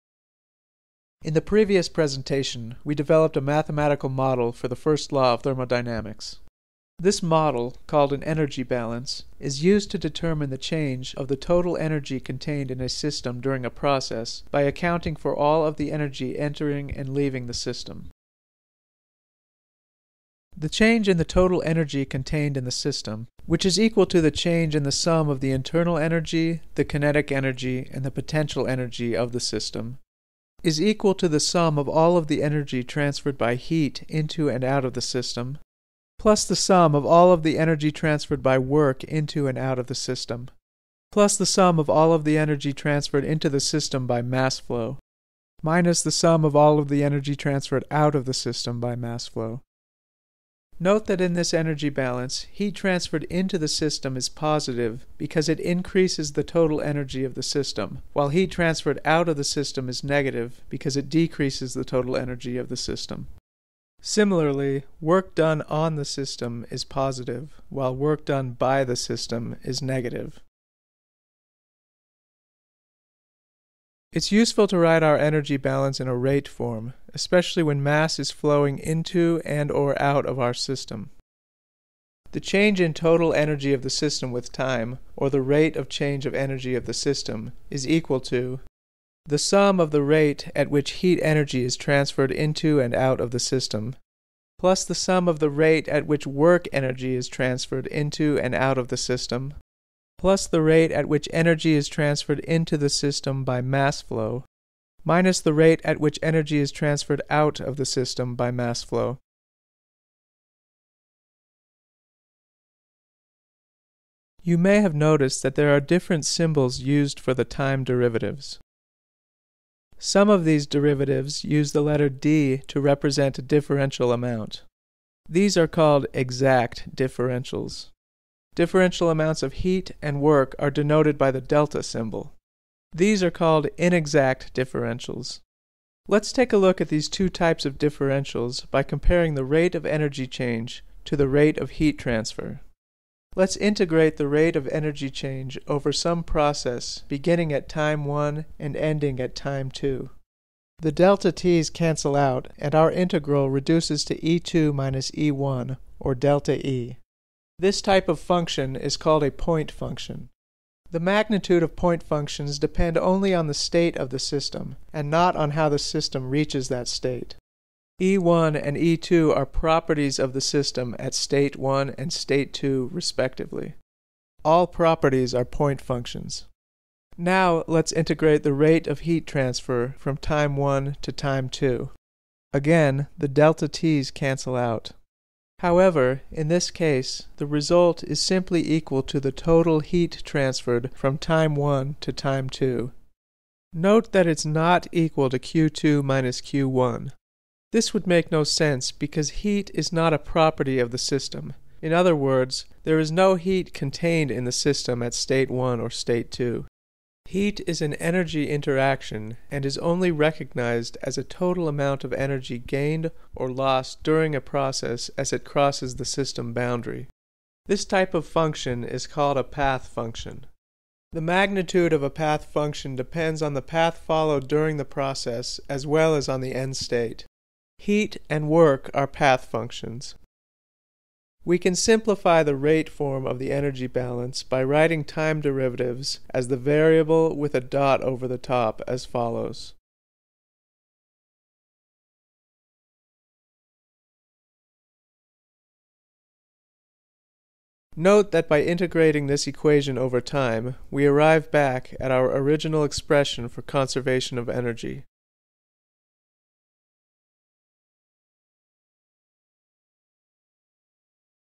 Thermodynamics Fundamentals: First Law, Part 4 - Energy Balance, Rate Form
This presentation is the last of four which discuss the first law of thermodynamics.